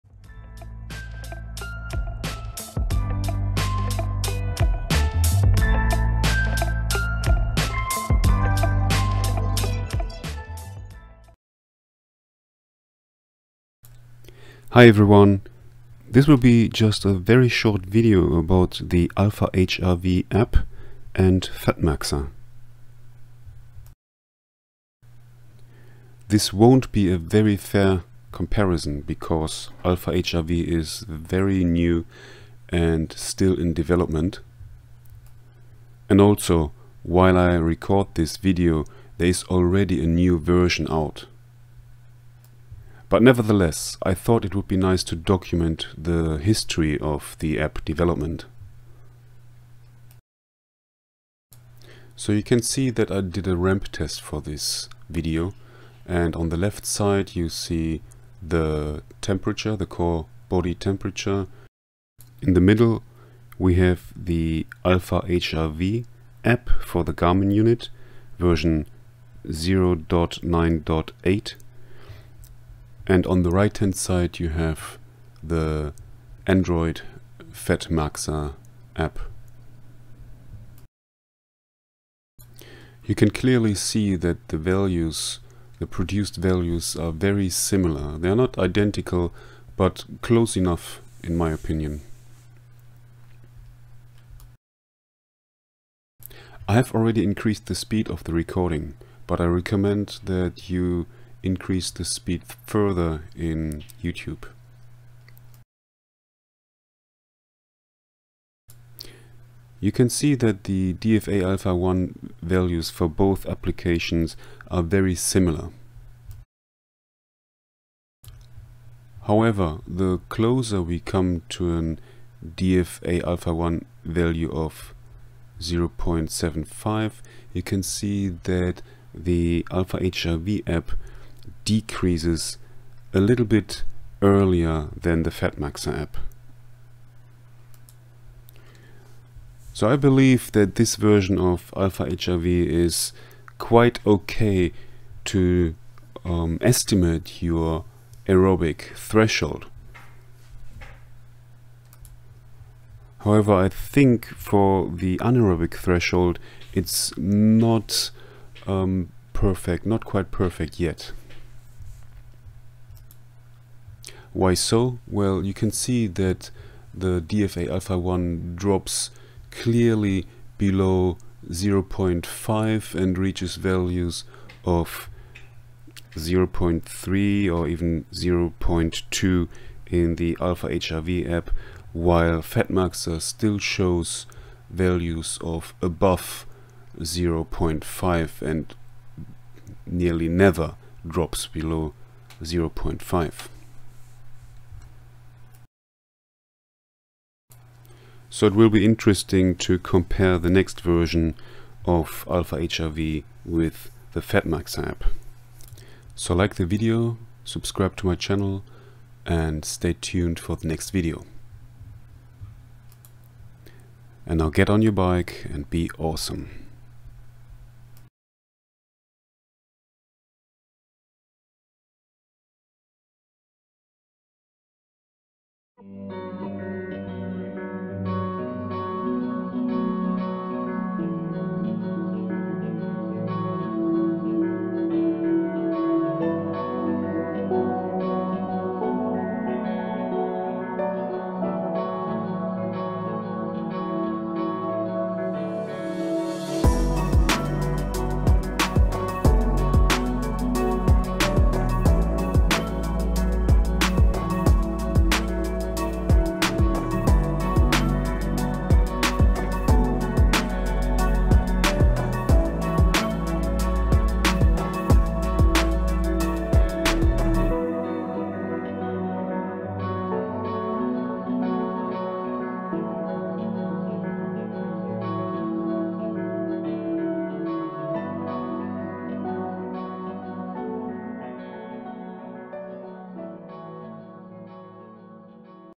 🔬⚗️👨‍🔬 🚴 AlphaHRV (v0.9.8) vs Fatmaxxer
In this video I make a very short comparison between the Garmin AlphaHRV data field and the Fatmaxxer app for Android.
The AlphaHRV app is still very new and while editing this video there have been 1-2 updates on it already. So take this short clip with caution. However, as you will see the two apps correspond with each other over a fairly wide range and I am sure they will soon be equal players in the HRV world.
I uploaded this video despite the fact that it's already 'out of date' since I thought that tracking its development can't be of harm.
I will soon deliver a comparison between the latest AlphaHRV version and Fatmaxxer.

'Alpha1 has been correlated by different authors with physiological thresholds and it is well accepted that alpha1 = 0.75 corresponds to aerobic threshold.'

'Alpha1 = 0.50 has been also related to anaerobic threshold, although in this case not so clear relation has been identified.'

'In order to use this DataField a heart rate monitor that provides HRV measurements is required. As accuracy in alpha1 identification depends on quality of the measurements, it is strongly recommended to use Polar H9 or H10 monitors that have been validated by different authors using existing HRV analysis platforms.'

alphaHRV connects using BLE or Ant+ to the HR sensor.